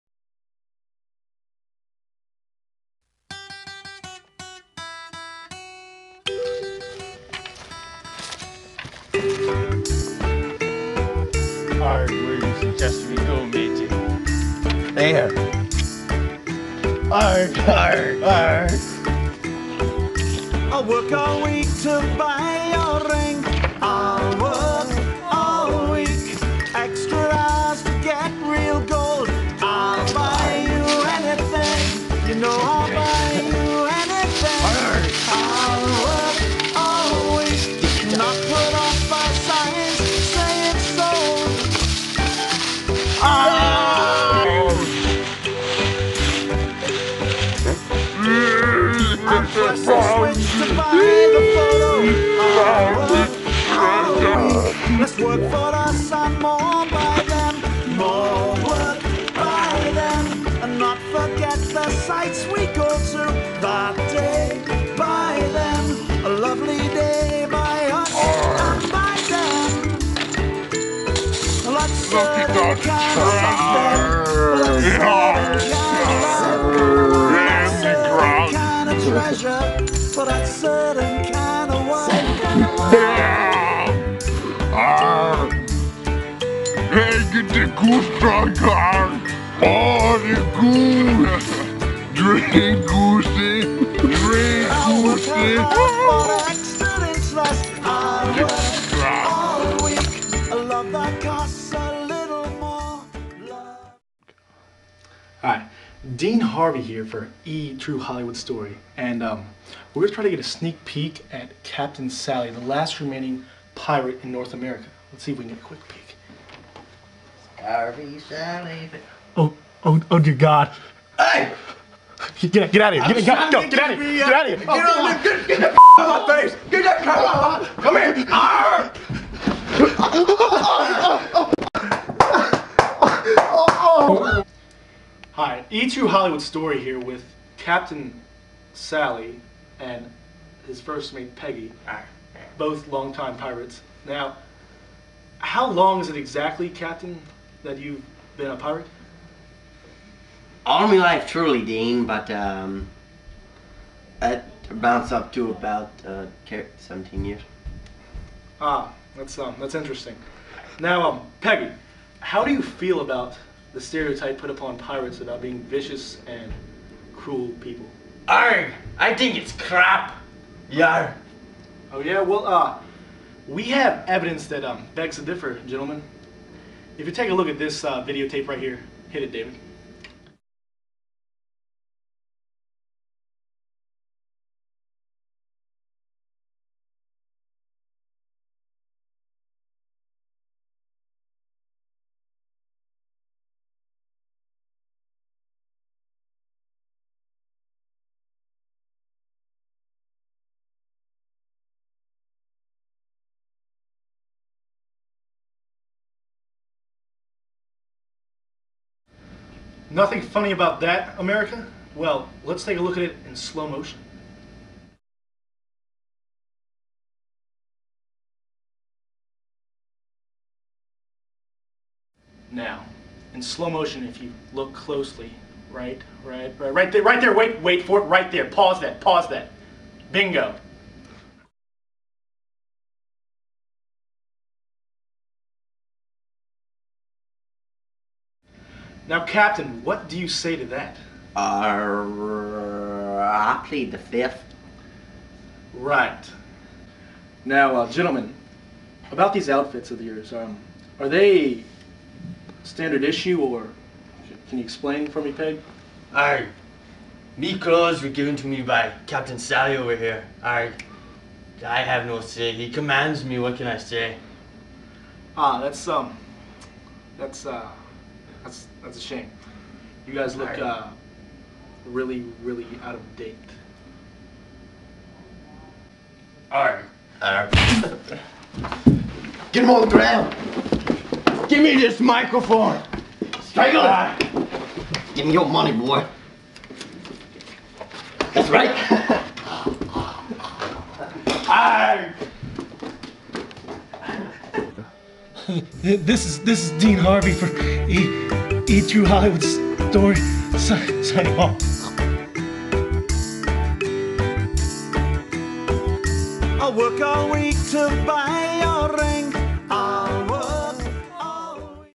True Pirate Story - Captain Sally and Peggy
Interview with Captain Sally and Peggy, the last remaining pirates in North America.